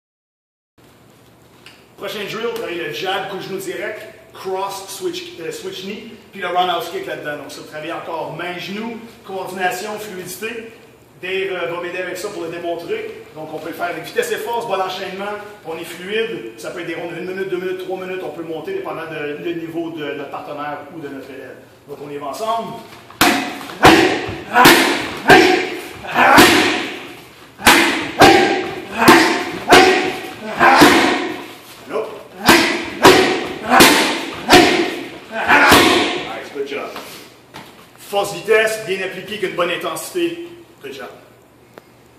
Gatineau martial arts/ Lethwei striking drills
Good drill to work hands/knee and kick combos.